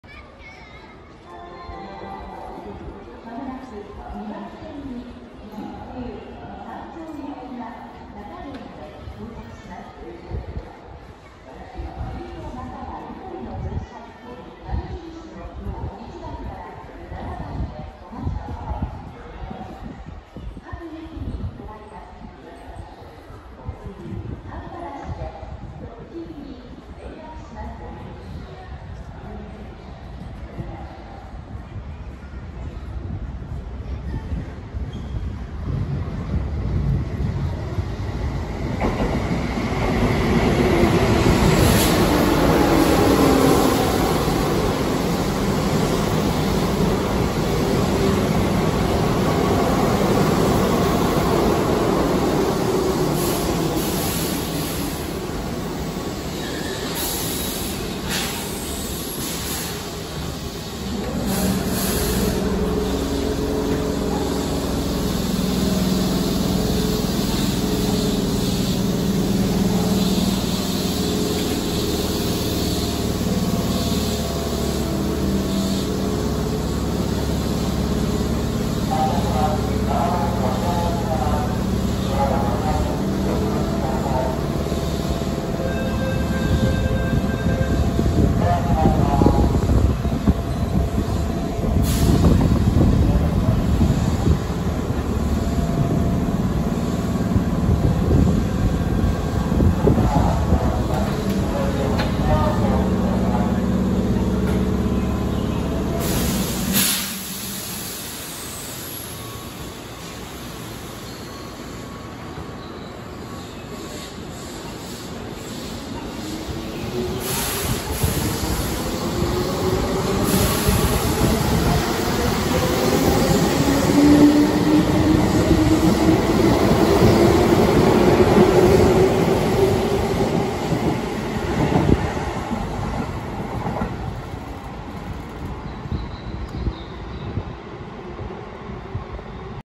京阪淀駅 2400系準急三条行きが発着
京阪淀駅で撮りました。2400系準急三条行きが発着します。
それにしても準急の三条行きは珍しい気がします…